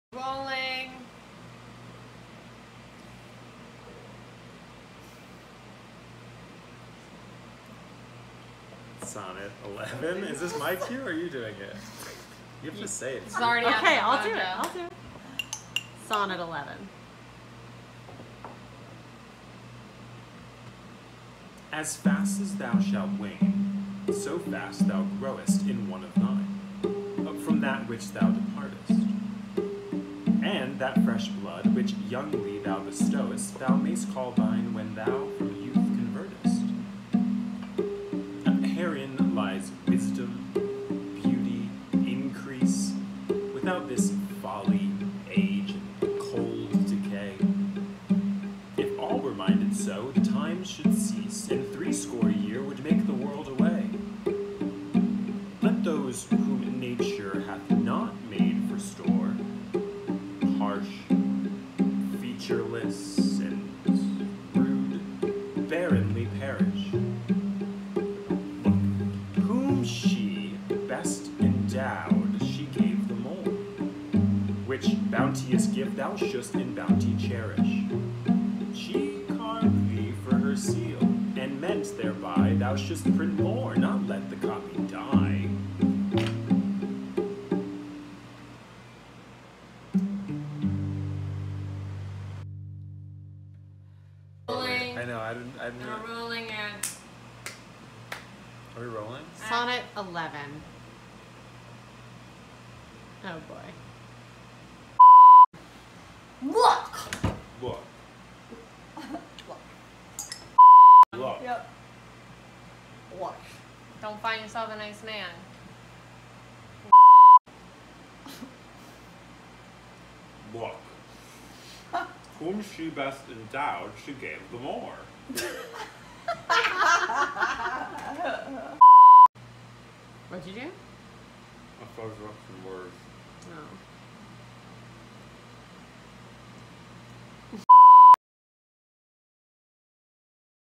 Sonnet 11
sonnet by Sheakespeare
read by Brandon Gomez
cello by Elisa Winter
featuring Carley Buckley and Katilin Wilson